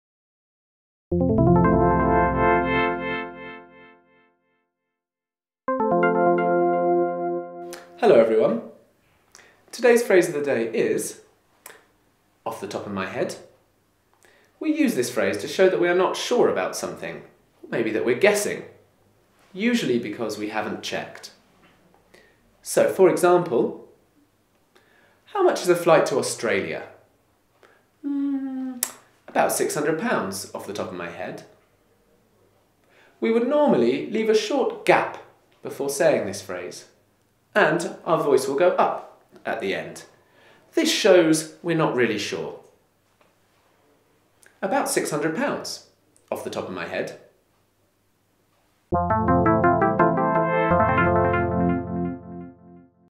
IH Bristol Phrase of the Day: Off the Top of My Head
IH Bristol Phrase of the Day
Learn a new English phrase everyday todays phrase is off the top of my head.